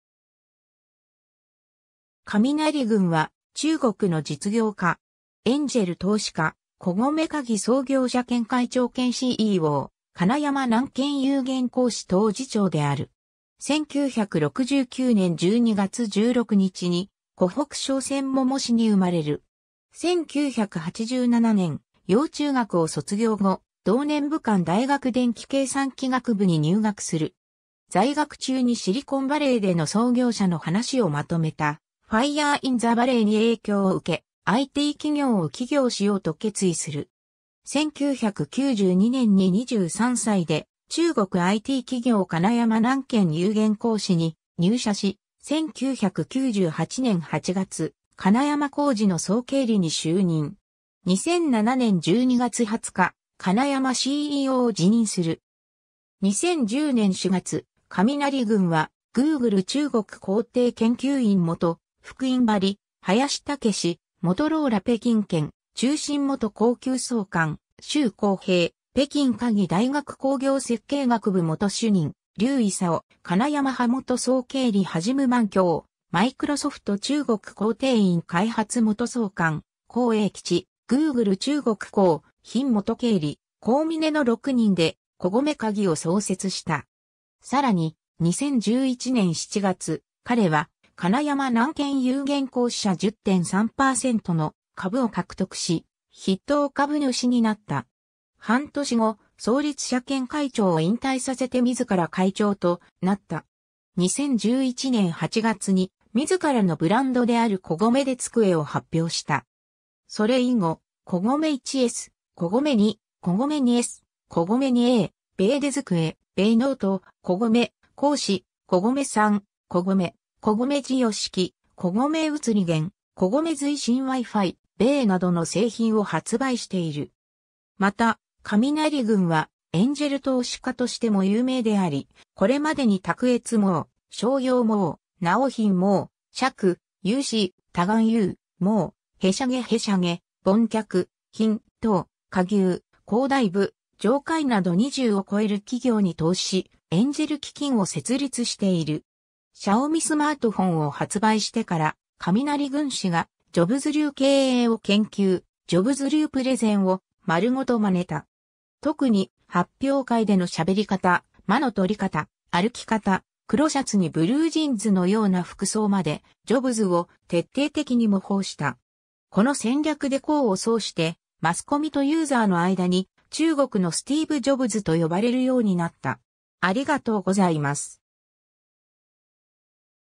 雷軍
雷 軍（らい ぐん、レイ・ジュン、拼音: Léi Jūn、1969年12月16日 - ）は、中国の実業家。
エンジェル投資家、小米科技創業者兼会長兼CEO、金山軟件有限公司董事長（取締役会長）である。
 1969年12月16日に湖北省仙桃市に生まれる。
1987年，沔陽中学（現湖北省仙桃中学）を卒業後、同年武漢大学電器計算機学部に入学する。
 在学中にシリコンバレーでの創業者の話をまとめた「fire in the valley」に影響を受け、IT企業を起業しようと決意する。
 1992年に23歳で中国IT企業金山軟件有限公司に入社し、1998年8月，金山公司の総経理（取締役社長）に就任。
 2007年12月20日、金山CEOを辞任する。
 2010年4月、雷軍はGoogle中国工程研究院元副院張林斌、モトローラ北京研发中心元高級総監周光平、北京科技大学工業設計学部元主任劉德、金山词霸元総経理黎万強、マイクロソフト中国工程院開発元総監黄江吉、Google中国高级产品元経理洪峰の6人で小米科技を創設した。
 さらに、2011年7月、彼は金山軟件有限公司社10.3％の株を獲得し、筆頭株主になった。
半年後、創立者兼会長（当時）を引退させて自ら会長となった。
 2011年8月に自らのブランドである小米手机を発表した。
それ以後、小米1S、小米2、小米2S、小米2A、红米手机、红米Note、小米盒子、小米3、小米电视、小米路由器、小米移动电源、小米随身wifi、米键などの製品を発売している。
 また、雷軍はエンジェル投資家としても有名であり、これまでに卓越网、逍遥网、尚品网、乐讯社区、UC优视 、多玩游戏网(欢聚时代)、拉卡拉、凡客诚品、乐淘、可牛、好大夫、长城会など20を超える企業に投資し、エンジェル基金を設立している。
 Xiaomiスマートフォンを発売してから、雷軍氏がジョブズ流経営を研究、ジョブズ流プレゼンを丸ごとまねた。
特に、発表会での喋り方、間の取りかた、歩き方、黒シャツにブルージーンズのような服装まで、ジョブズを徹底的に模倣した。
この戦略で功を奏して、マスコミとユーザーの間に「中国のスティーブ・ジョブズ」と呼ばれるようになった。